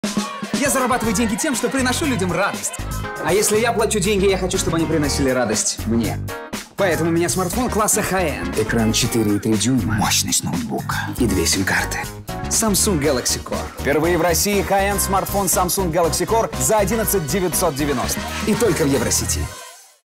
Тимур Родригез представляет Samsung Galaxy Core в новой рекламе Евросети
Евросеть объявляет о старте рекламной кампании нового смартфона Hi-End класса Samsung Galaxy Core с двумя SIM-картами. Новинка представлена в России впервые и продается только в салонах и интернет-магазинах Евросети.

Одним из преимуществ Samsung Galaxy Core является возможность одновременной поддержки двух SIM-карт. Смартфон Hi-End класса* обладает мощным двухъядерным процессором с частотой 1,2 ГГц и работает на популярной платформе Android 4.1. Jeally Bean, что обеспечивает высокую производительность аппарата.

Лицом новой рекламной кампании «Евросеть» стал известный российский шоумен, теле- и радиоведущий, финалист популярного шоу «Один в один» Тимур Родригез.
Рекламная кампания смартфона Samsung Galaxy Core пройдет с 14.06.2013 по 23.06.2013 на федеральных телеканалах: Первый, РЕН ТВ, ТВ-3, Ю-ТВ, MTV, 2*2, 5 Канал, Дисней, Россия1, СТС, Перец, НТВ, ТНТ.

Состав творческой группы:
Евросеть:
Наталья Дубик -- директор по рекламе
Мария Клеймёнова  - ведущий менеджер по маркетинговым коммуникациям

Samsung:
Ирина Васильева -- руководитель группы COOP маркетинга
Екатерина Виткарь - менеджер группы COOP маркетинга

Fresh Production:
Тигран Бежанов -- режиссер
Марат Адельшин -- оператор
Мишель Шениара -- продюсер
Алина Яцык -- продюсер

Samsung Galaxy Core -- это мощный и функциональный смартфон с двухъядерным процессором 1,2 ГГц, 8 Гб встроенной и 1 Гб оперативной памяти. Дисплей диагональю 4,3'' с разрешением экрана 480х800 отлично подходит для чтения текста, просмотра веб-страниц, фотографий и видео. Емкость аккумулятора 1800 мАч позволяет пользоваться смартфоном до 9 часов в режиме разговора, до 8 часов при просмотре видеофайлов или активном серфинге в интернете. Камера 5 Мпикс оснащена светодиодной вспышкой и поддерживает автоматическую фокусировку. Помимо технических преимуществ смартфон обладает также такими интеллектуальными функциями и сервисами, упрощающими пользование устройством, как «Интеллектуальное ожидание», «Интеллектуальное оповещение», «Лучший кадр» и голосовое управление S Voice. Samsung Galaxy Core представлен в салонах компании и интернет-магазине «Евросети» в синем и белом цветовых решениях по цене 11 990 рублей**.

* Hi-End класс -- лидирующий, высочайший класс технических устройств.
**Цена действительна на момент публикации.